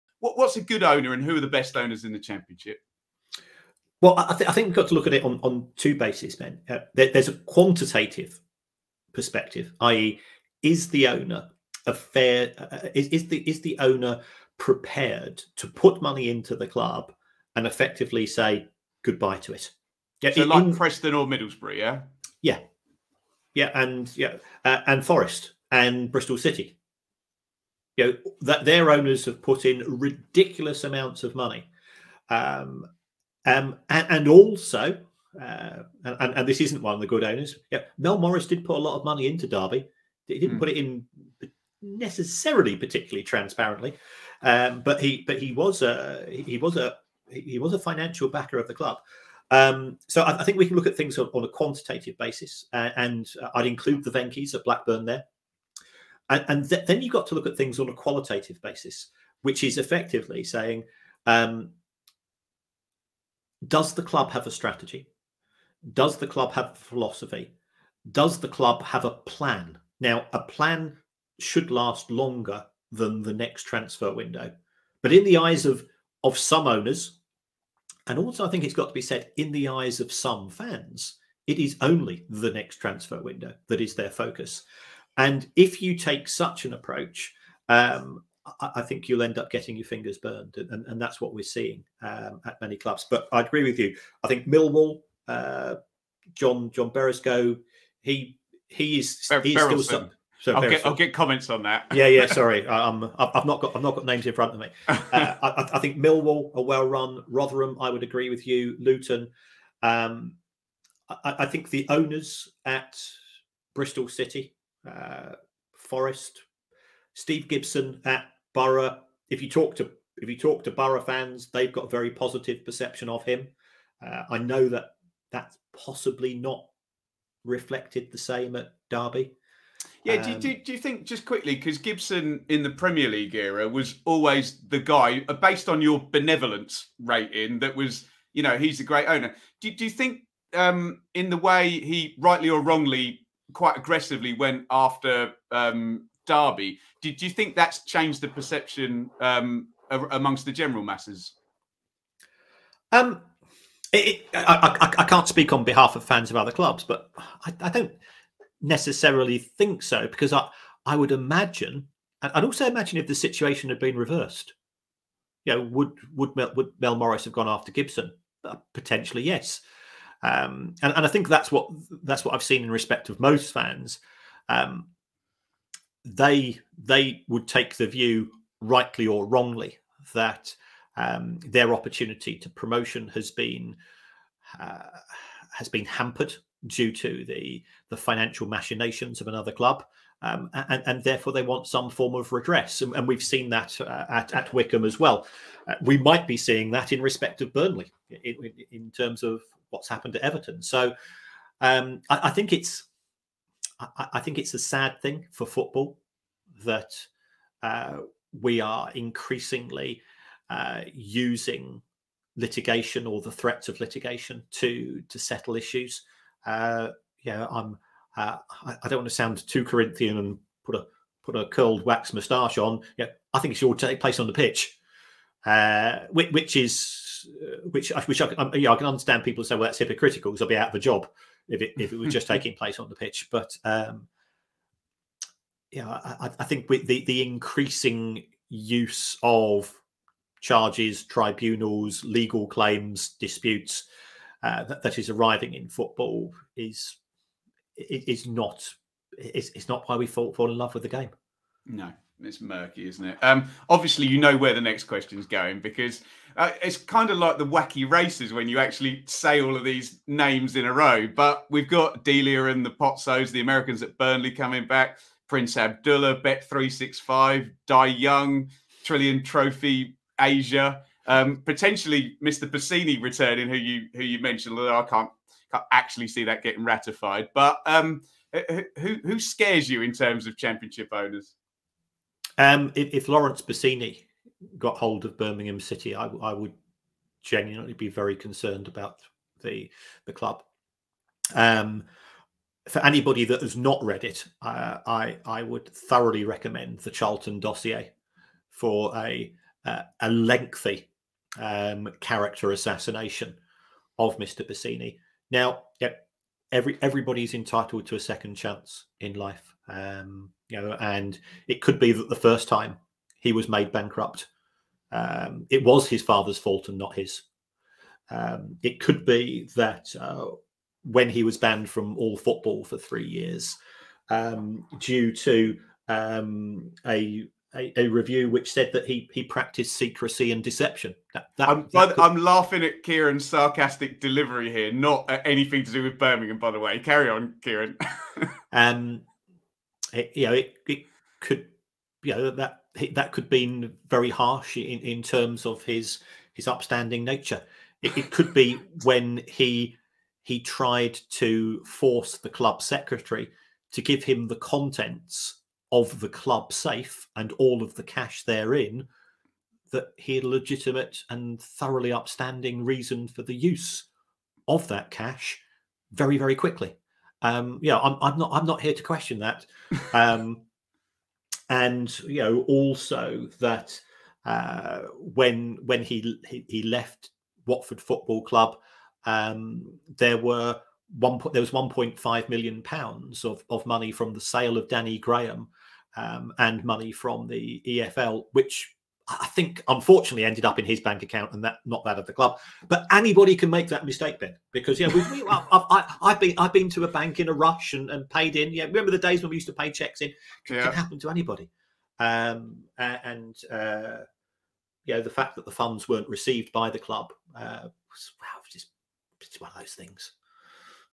Who are the BEST AND WORST OWNERS in the Championship? w/Kieran Maguire
I caught up with Price Of Football author Kieran Maguire and asked him who the best and worst owners in the Championship are!

Full interview available soon!!